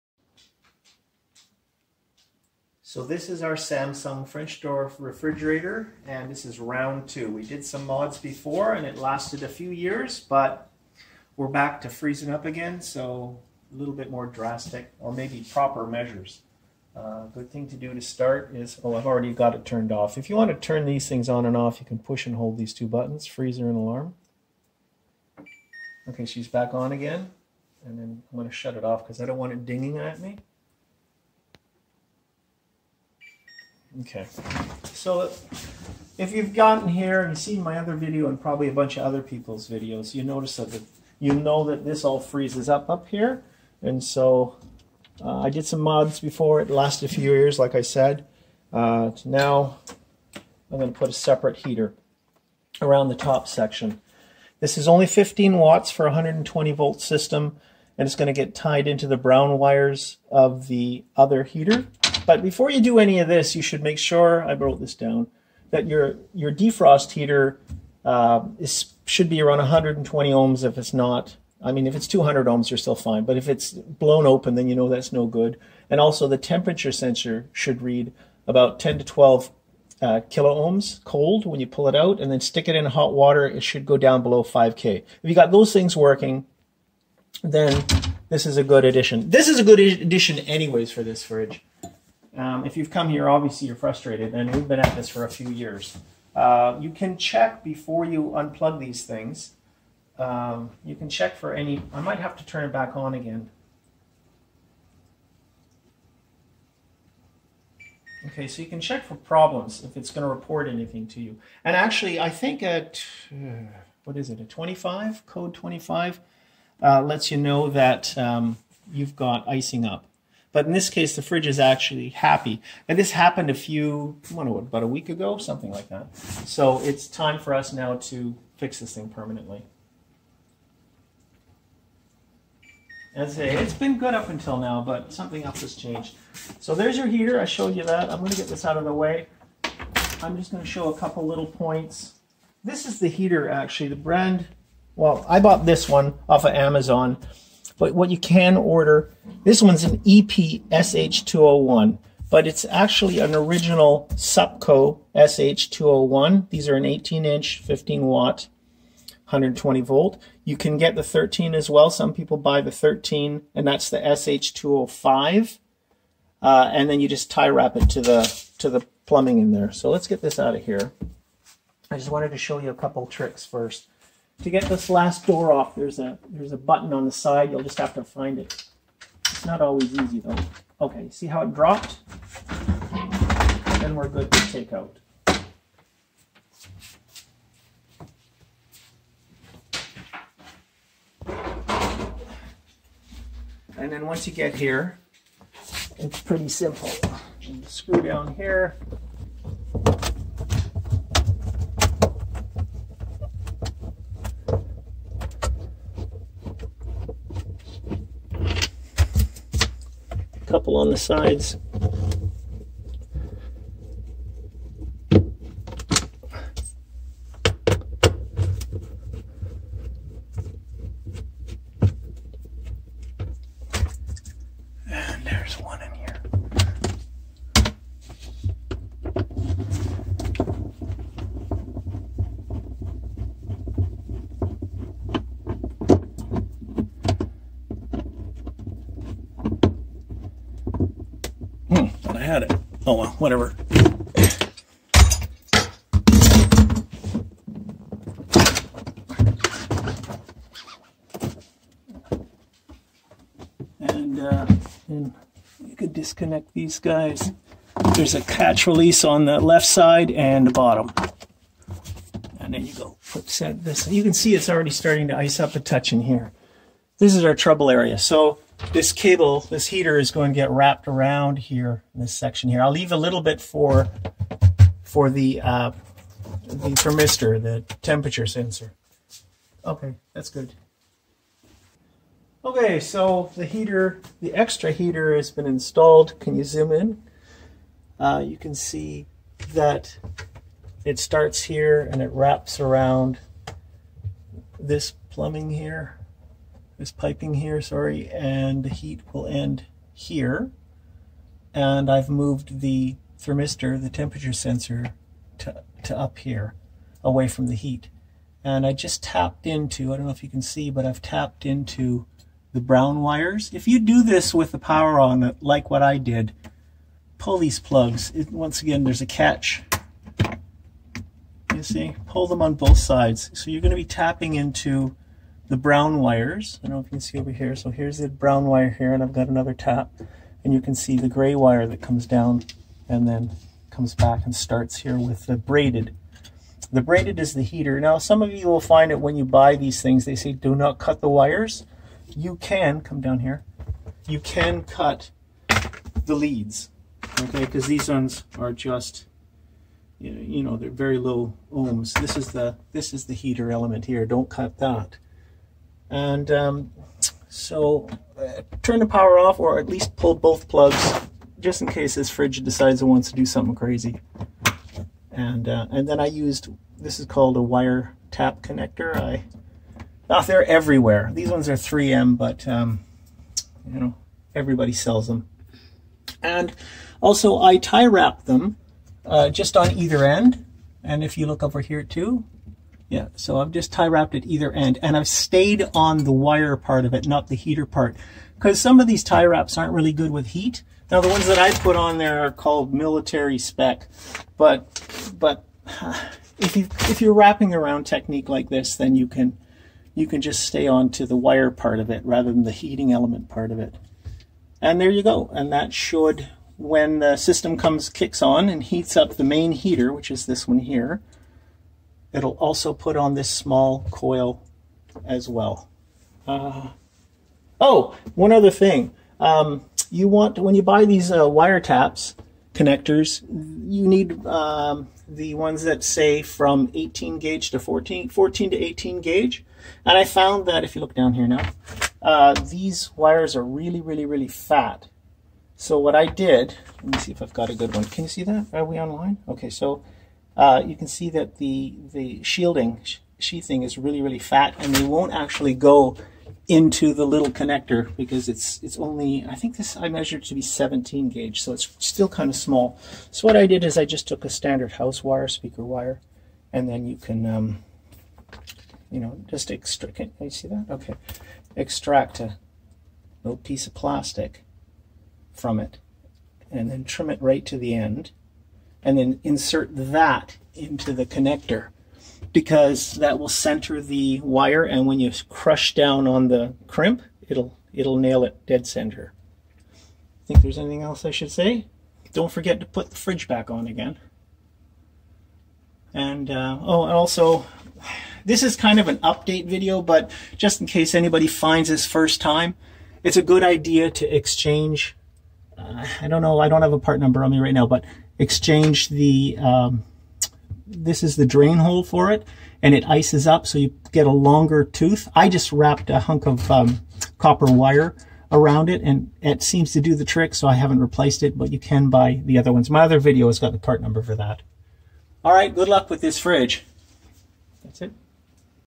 Samsung French door refrigerator heater modification for icing issue
Round 2
We got a few good years out of the original modifications made but the icing issue is back. Time for a better fix.

heating wire:
Supco SH201, 18" 120vac 15W (wrapped around)
or
Supco SH205 13" 120vac 15W (ty wrapped)

wire taps are 14-18 gauge

temperature sensor:
 over 10k ohm iced
 less than 5k ohm hot water

main defrost heater ~120 ohm
(there is probably a thermo fuse along the way to consider as well)

longer Samsung clip drain evaporator
 DA61-06796A

get Error Codes before powering down AC:
 press and hold Fridge and Freezer for 8 sec
 for an iced up condition, error code 25E will probably be stored

turn off cooling for servicing (and back on):
 press and hold Freezer and Alarm to set to "0 FF"